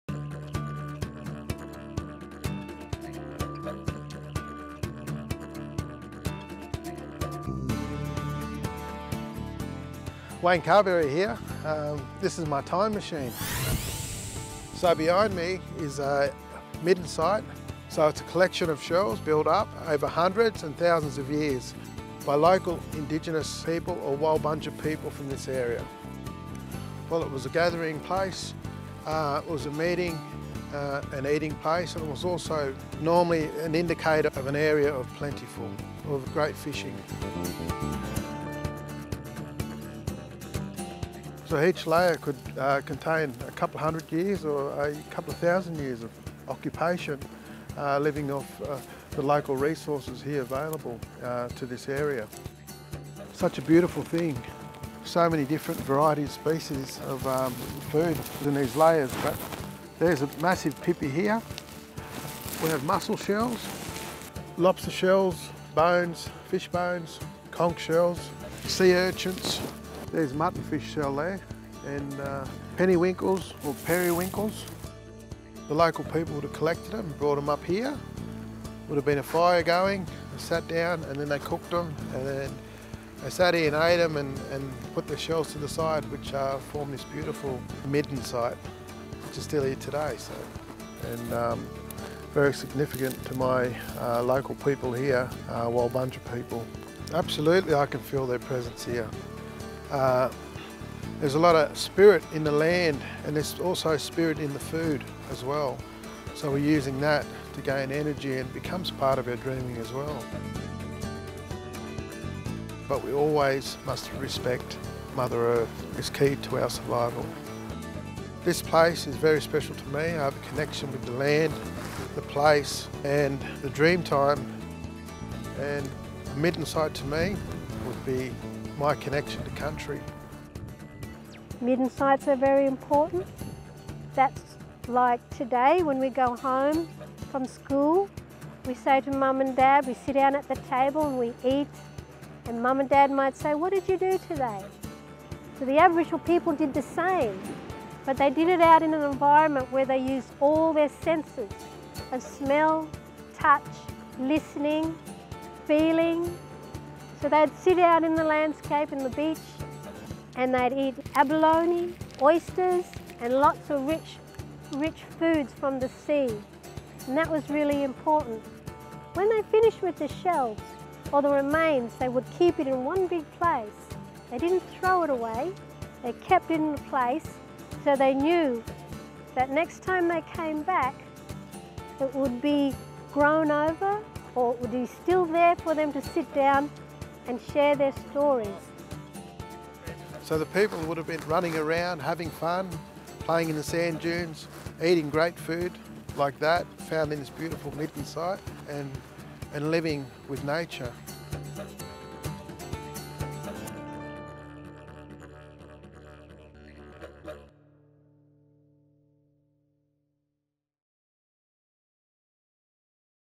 What's in my Midden?
Let Walbunga man Wayne Carberry take you back in time to find out what's in the midden. These sites tell us what Aboriginal people have eaten on the coast over thousands of years as well as the tools that they used to harvest and prepare food.

This is a South Coast Sea Country video produced by NSW DPI Fisheries with funding from the NSW Marine Estate Management Strategy.